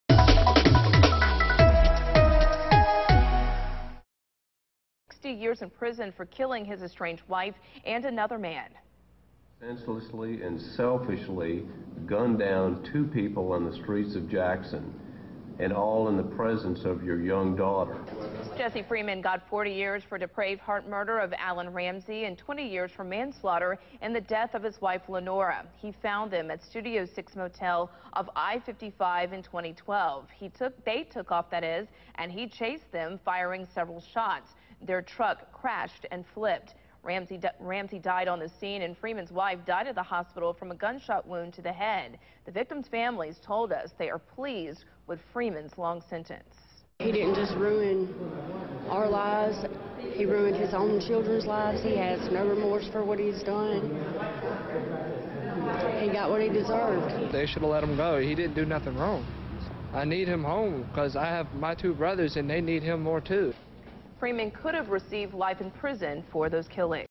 Freeman sentenced in double slaying
A judge sentenced a man today to 60-years in prison for killing his estranged-wife and another-man.